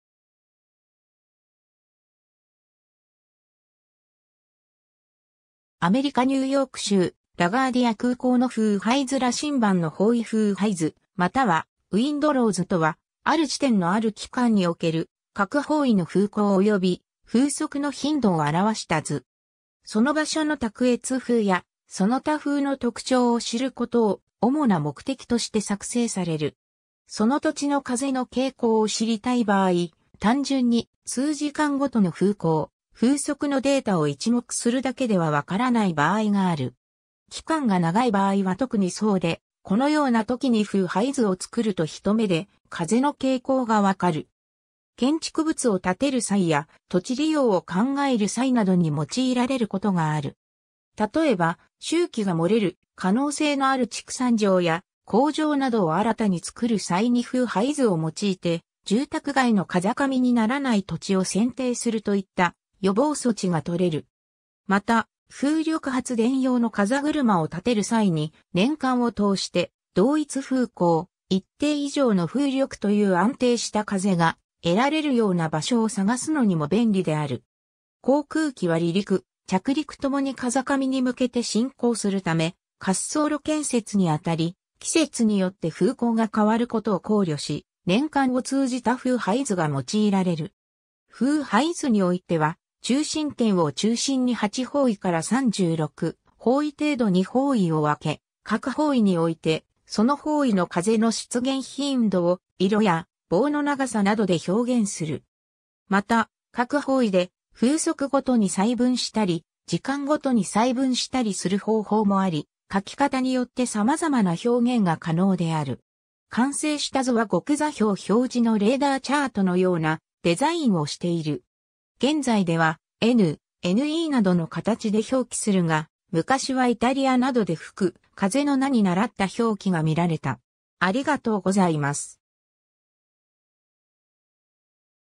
風配図
アメリカ・ニューヨーク州ラガーディア空港の風配図 羅針盤の方位 風配図（ふうはいず）またはウインドローズ(wind rose)とは、ある地点のある期間における、各方位の風向および風速の頻度を表した図。
その場所の卓越風や、その他風の特徴を知ることを主な目的として作成される。
 その土地の風の傾向を知りたい場合、単純に数時間ごとの風向・風速のデータを一目するだけでは分からない場合がある。
期間が長い場合は特にそうで、このようなときに風配図を作ると一目で風の傾向が分かる。
 建築物を立てる際や土地利用を考える際などに用いられることがある。
例えば、臭気が漏れる可能性のある畜産場や工場などを新たに作る際に風配図を用いて、住宅街の風上にならない土地を選定するといった予防措置が取れる。
また、風力発電用の風車を立てる際に、年間を通して同一風向・一定以上の風力という安定した風が得られるような場所を探すのにも便利である。
航空機は離陸・着陸ともに風上に向けて進行するため、滑走路建設にあたり、季節によって風向が変わることを考慮し年間を通じた風配図が用いられる。
 風配図においては、中心点を中心に8方位～36方位（大体16方位が多い）程度に方位を分け、各方位において、その方位の風の出現頻度を色や棒の長さなどで表現する。
また、各方位で、風速ごとに細分したり、時間ごとに細分したりする方法もあり、書き方によってさまざまな表現が可能である。
完成した図は極座標表示のレーダーチャートのようなデザインをしている。
 現在では、N、NEなどの形で表記するが、昔はイタリアなどで吹く風の名に倣った表記がみられた。